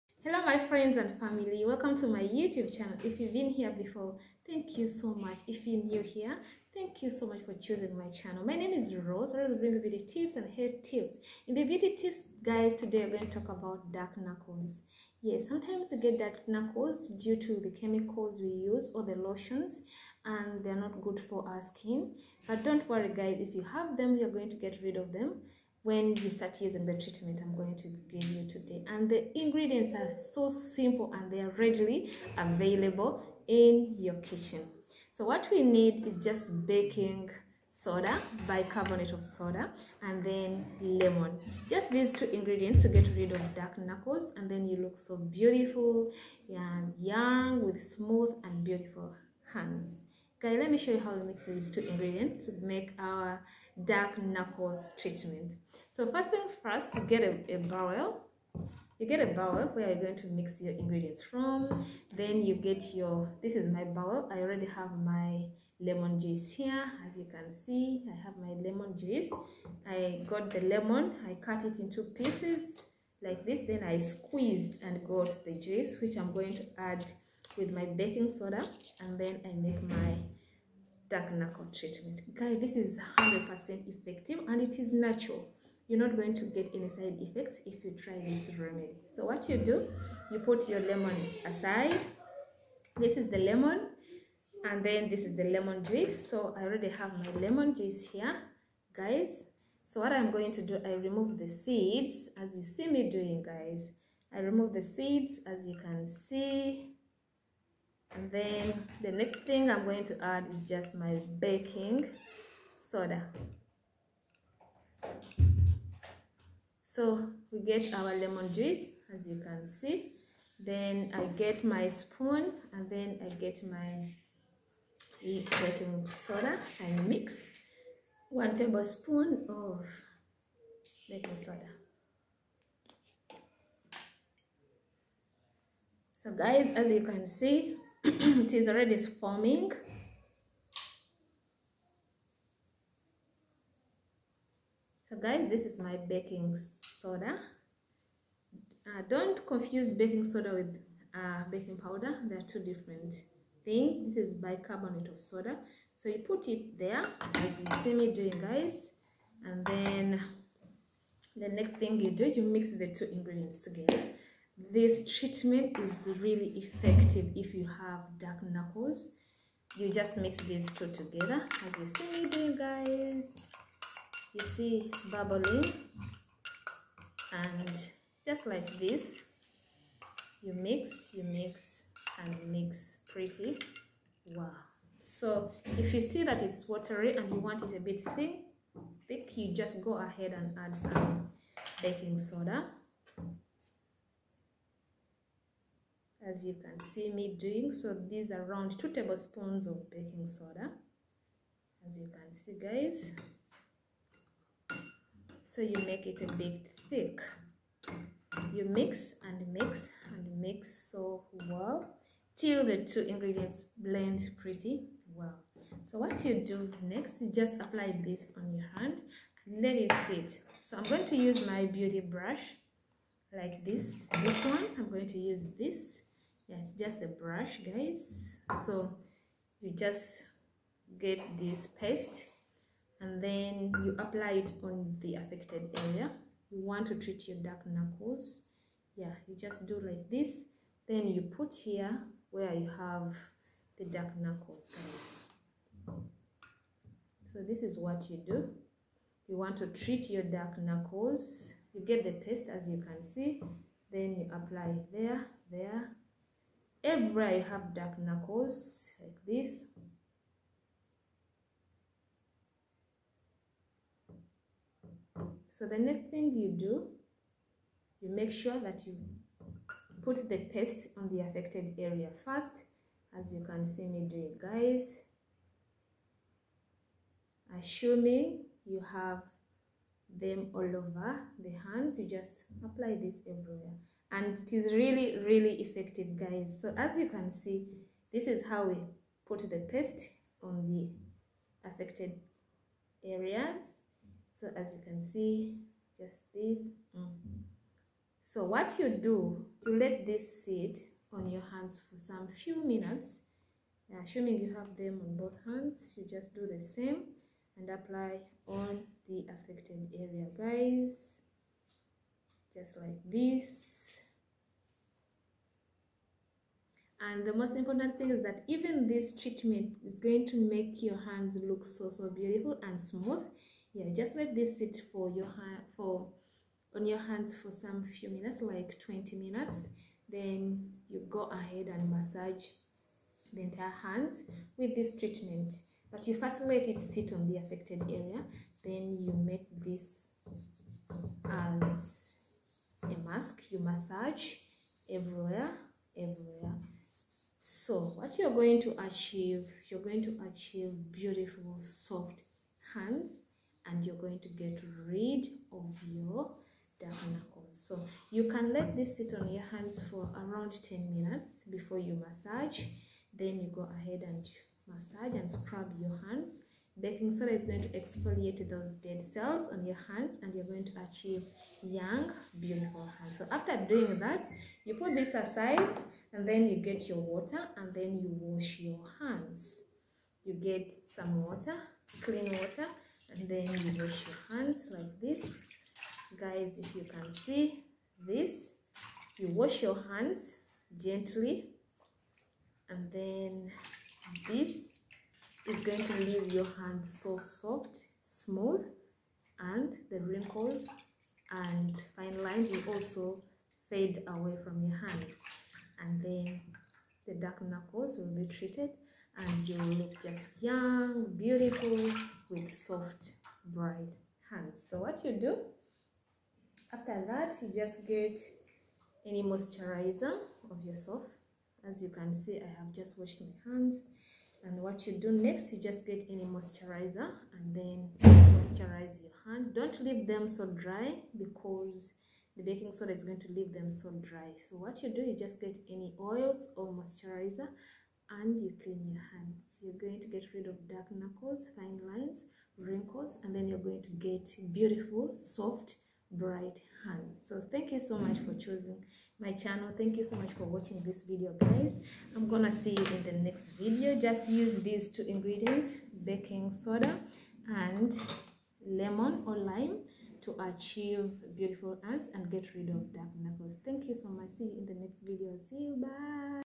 HOW TO GET RID OF DARK KNUCKLES NATURALLY AT HOME./DIY.
Hi Family,
In this video, I am showing you how to get rid of dark knuckles and rough hands naturally with Lemon and Baking Soda.

Ingredients
Lemon juice 2 tbspn
Baking Soda 2 tbspn

Just mix the above ingredients together to form a paste.
Apply the paste to the affected area, then massage your hands for few minutes. The dark knuckles will clear, the hands will become so smooth, bright and soft.

Rose